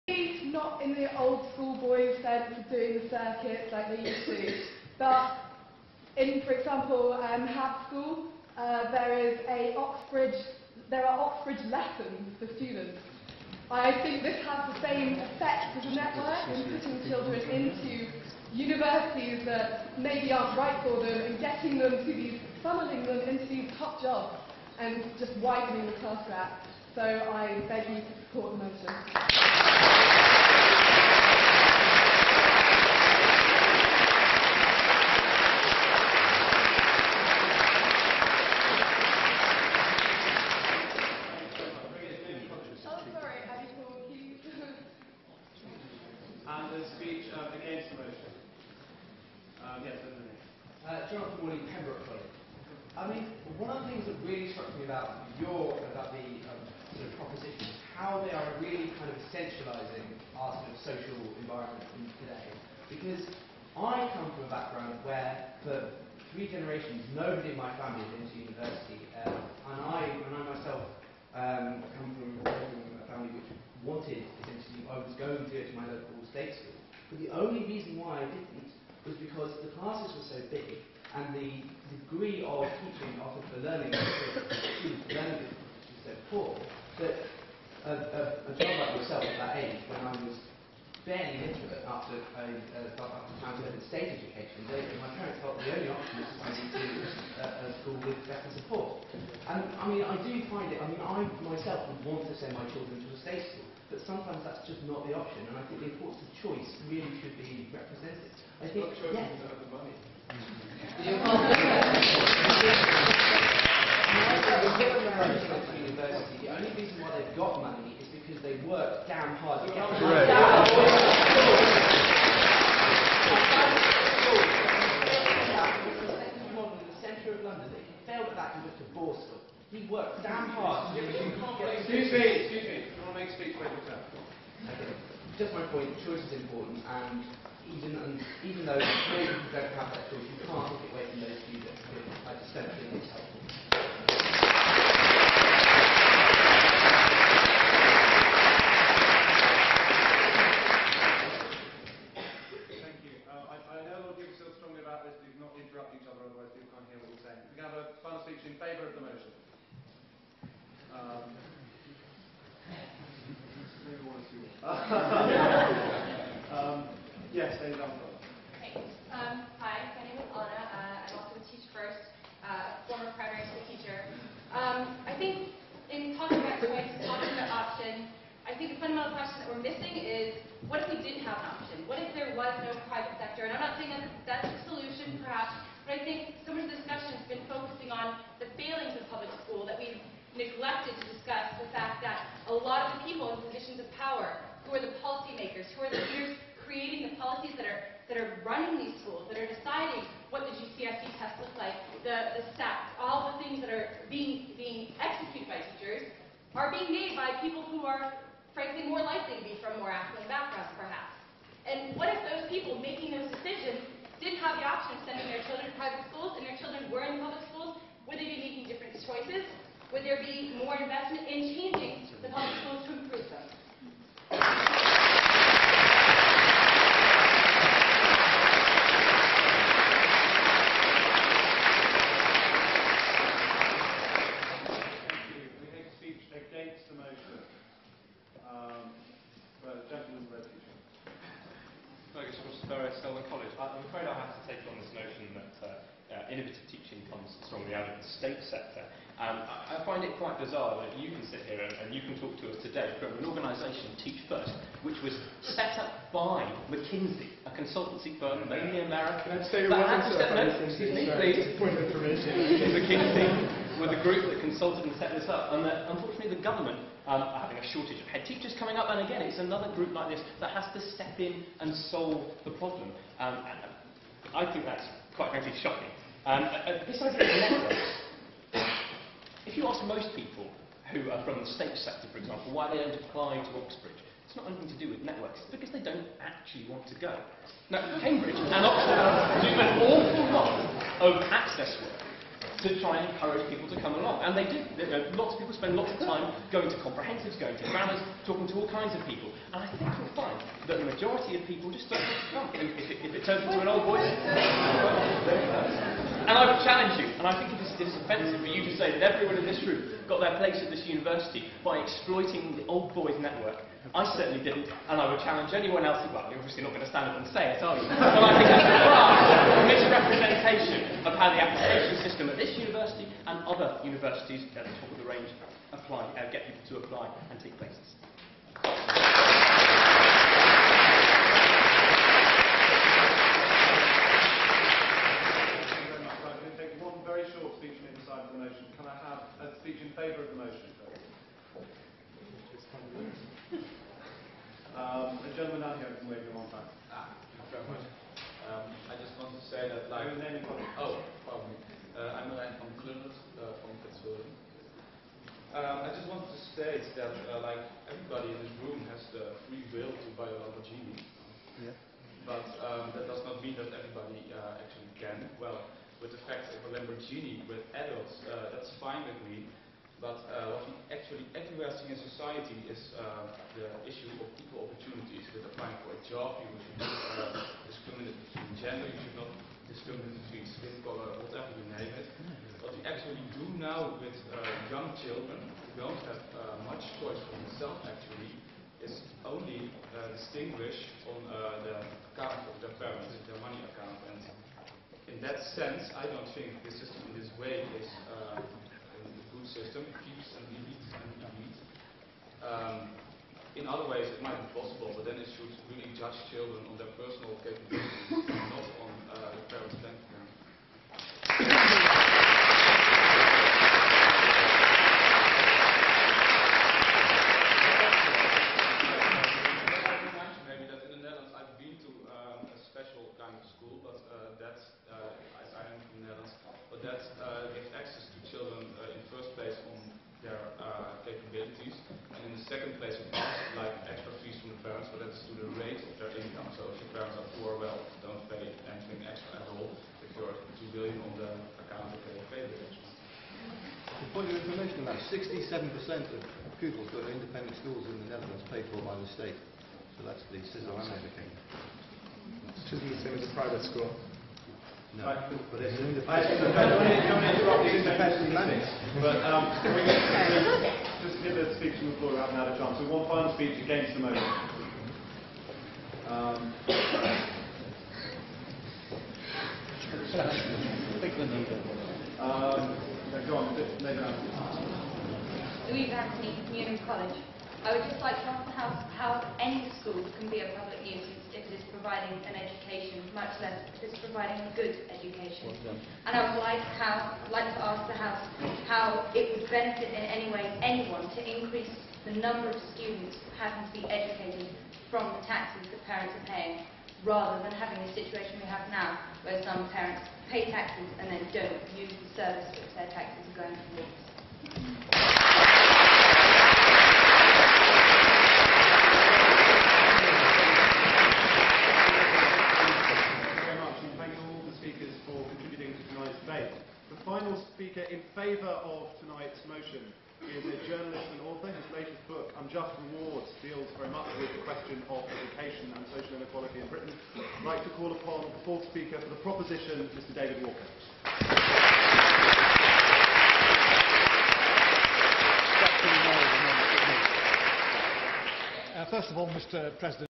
(Part 10 of 13) This House Believes that Private Schools are a Public Nuisance
Recorded at the Cambridge Union on 11/20/08. Featuring Michael Pyke, Fiona Millar, Brett Wigdortz, David Walker, Admiral Chris Parry, Elisa Haining, Dr. David Green and Jack Watson.